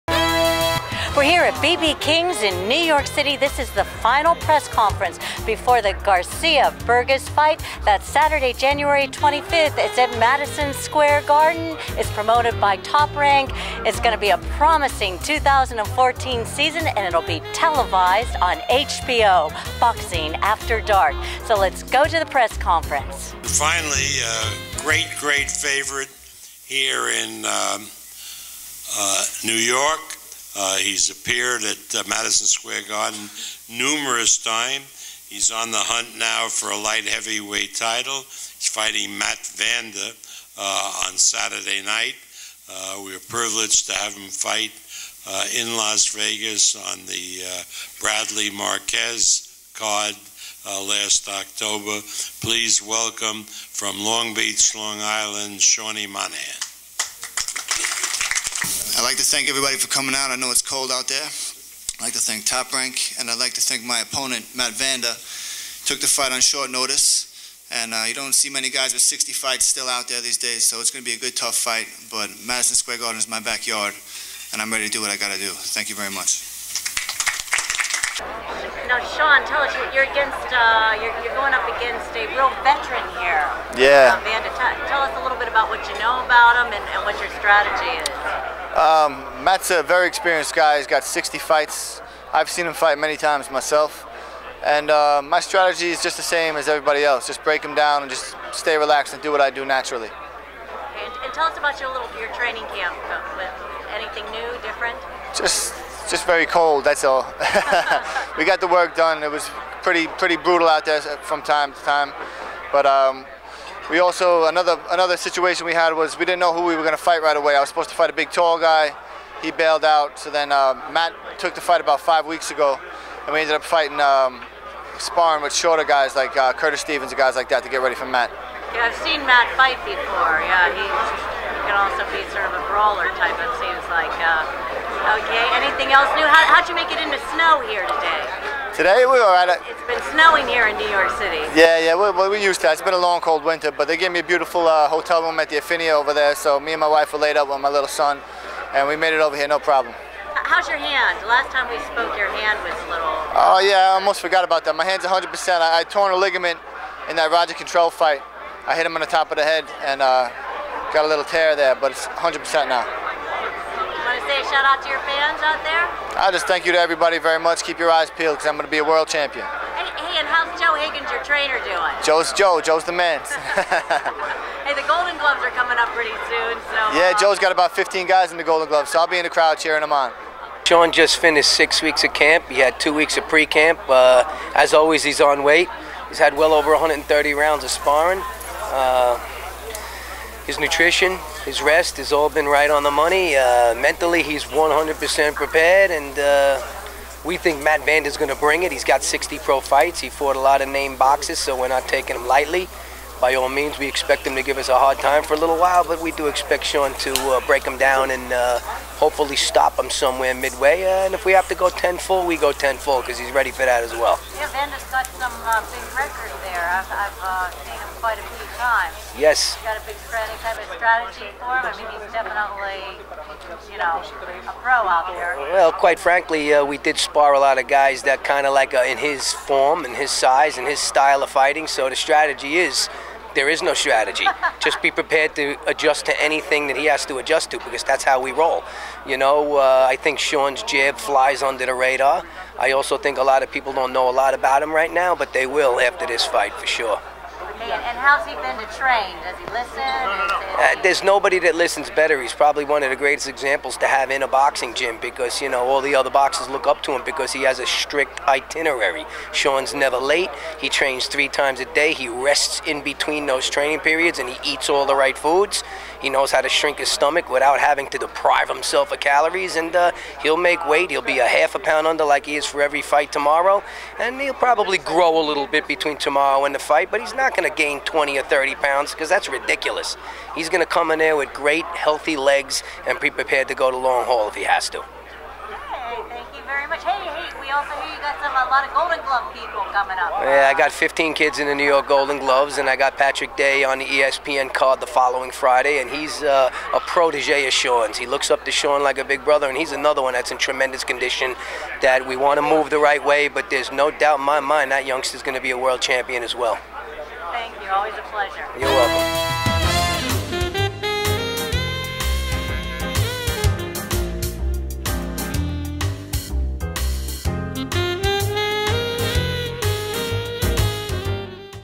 Sean Monaghan vs. Matt Vanda interview at Garcia vs. Burgos Final Presser NYC 1/23/14
WBC Continental Americas Light Heavyweight Sean Monaghan (19-0, 12 KOs) faces Matt Vanda (45-15, 24 KOs) on Saturday, January 25th on the Garcia vs. Burgos undercard at Madison Square Garden.
Monaghan captured the title in June 2012, knocking out Romaro Johnson in the eighth round, and has successfully defended it twice.  Monaghan enters this fight having won four of his last six fights by stoppage and world-rated No.6 by the IBF and No. 1 by the WBA.  He will face Matt Vanda (45-15, 24 KOs), of St. Paul, MN., in a 10-round light heavyweight battle.